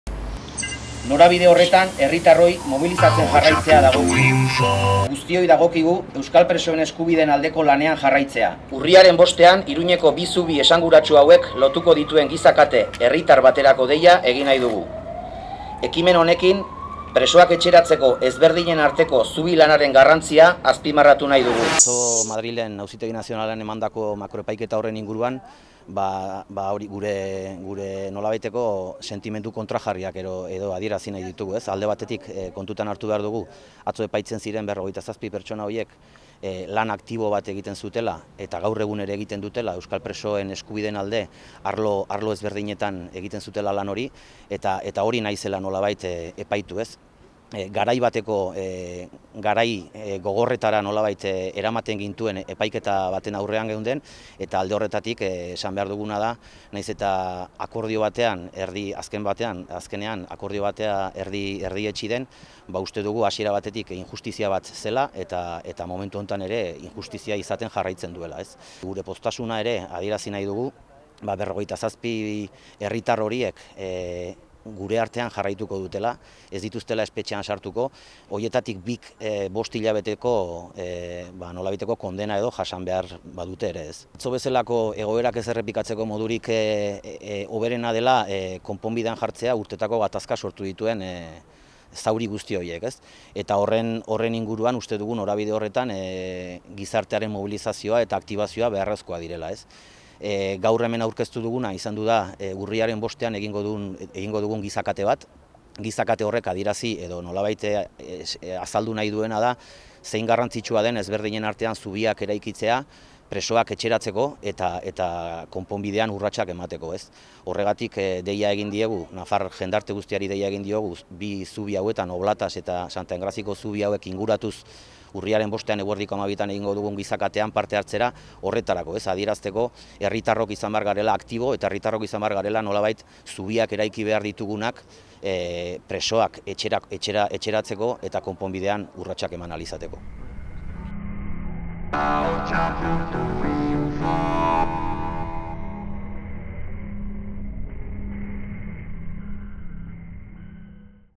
Presoak etxeratzeko "zubiak eraiki" behar direla dio Sarek eta giza kate bat antolatu du Iruñean
Urriaren 5erako giza katea antolatu du Sarek Iruñean, eta herritarrak aktibatzera zein mobilizazioan parte hartzera dei egin dute.

Herritarrek "presoak etxeratzeko zubi" izan behar dutela uste du Sare Herritarrak, "konponbidearen zein elkarbizitzaren alde zubiak eraikitzeko prest dauden herritar aktibo". Ahaleginak ugaritzeko garaia dela uste dute, besteak beste, euskal presoak etxeratzeko helburua lehenago erdiesteko. Bide horretan, giza katea antolatu dute Iruñean, urriaren 5erako.

Zubiak eraikitze horretan, bi zubi lotzea dute helburu. Oblataseko eta Santa Engraziko zubiak elkartu nahi dituzte, eta, horretarako, herrietan zein auzoetan jendea aktibatzeko lanari lotzeko eskatu dute. Zubiak presoak etxeratzeko leloarekin egingo duten mobilizazioa eguerdian izango da, eta "ezberdinen arteko zubi lanaren garrantzia" azpimarratu nahi dute.

"Iritzi kontrajarriak" 11/13 auziko epaiari buruz
Sareko kideek poza agertu dute atzo Madrilen hasi eta bukatu zen epaiketaren akordioaren inguruan. Poza, 45 epaitu ez direlako egongo "salbuespenezko espetxe politikaren atzaparretan". Baina, bestetik, "larria" iruditzen zaie epaiak "mendeku gosea" izatea. "Injustizia baten aurrean gaude".